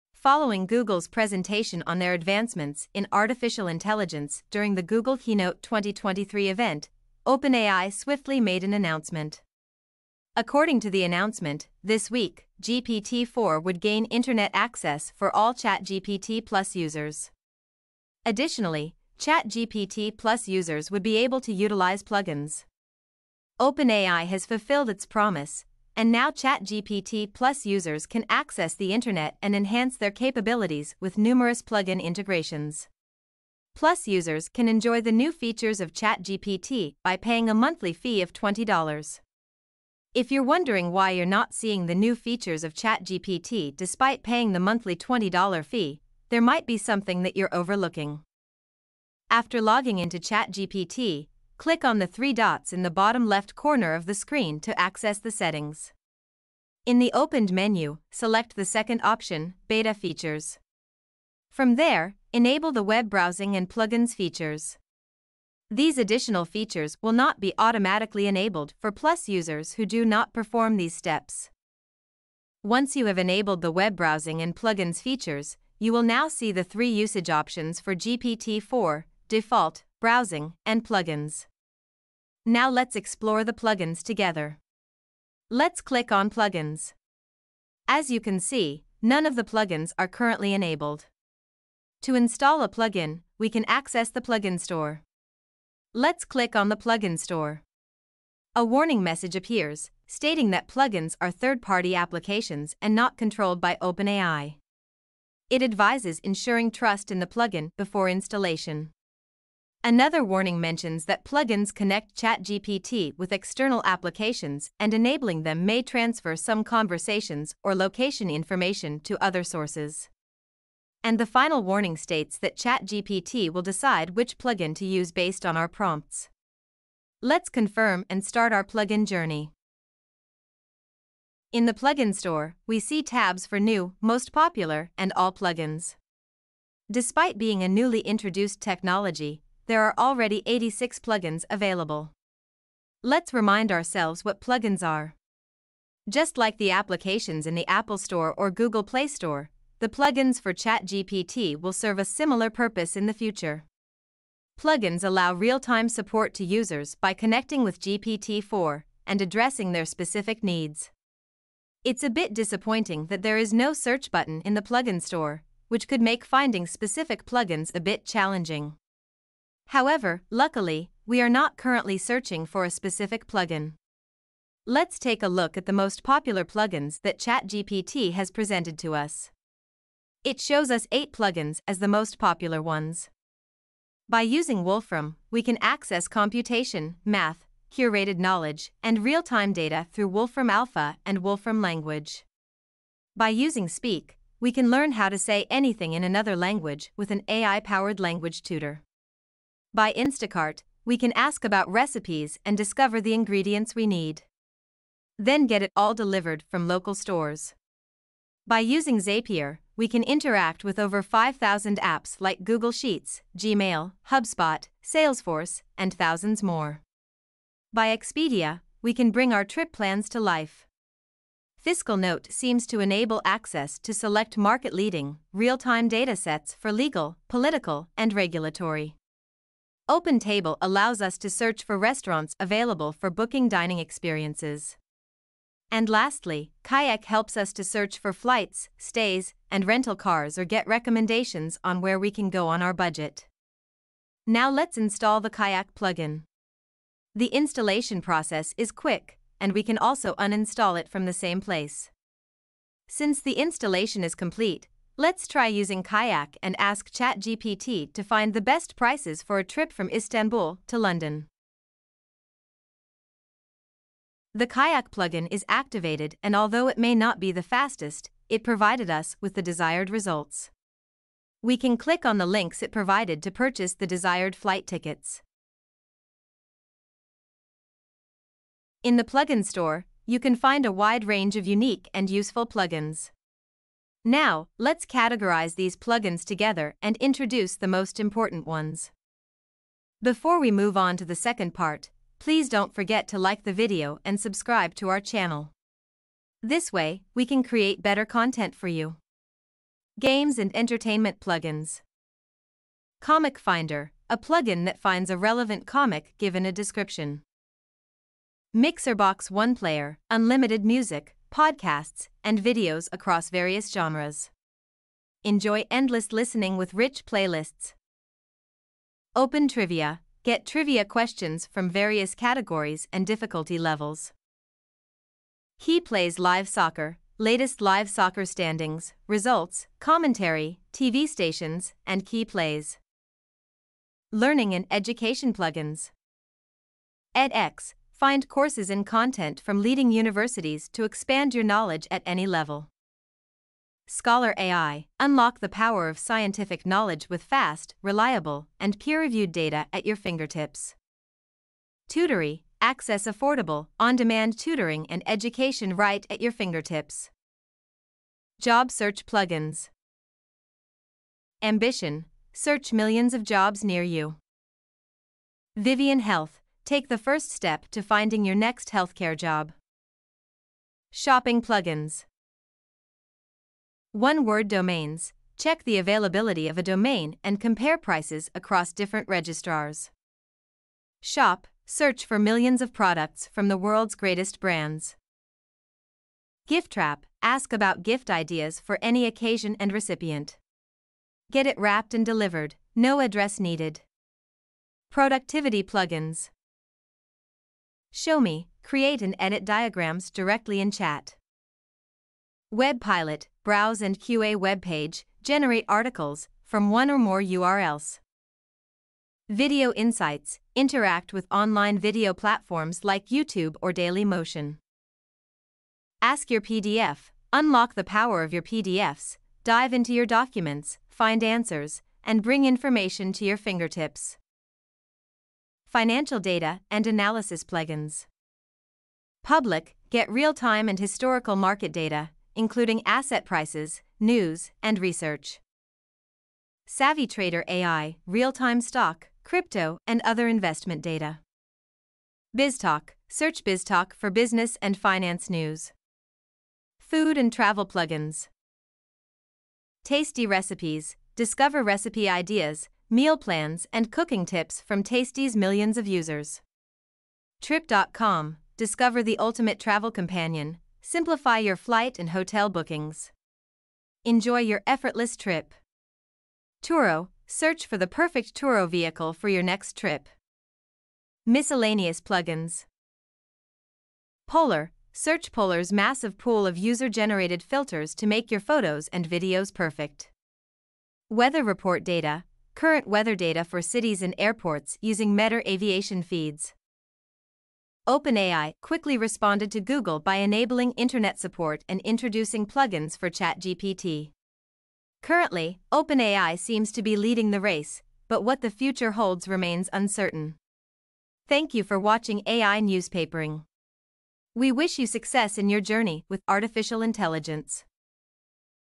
Game-Changing World of ChatGPT Plugins and Web Browsing
OpenAI regained its leadership shortly after Google's advancements in the field of artificial intelligence. ChatGPT Plus users now have access to ChatGPT's internet connection, and they can leverage the power of GPT-4 in real time with plugins tailored to specific topics.

In our video, we demonstrated how to enable internet and plugin access for ChatGPT, and we explored the most popular plugins. Additionally, we categorized all the plugins and introduced some noteworthy ones from each category.

This way, we can create better content for you. Wishing everyone success on their journey with artificial intelligence!